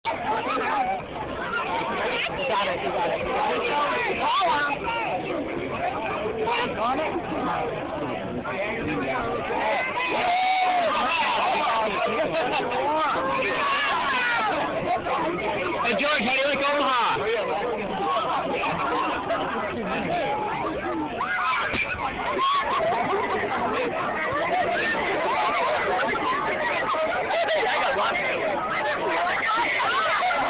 George Clooney in Omaha
I went to lunch downtown and low and behold, George Clooney was filming across the street.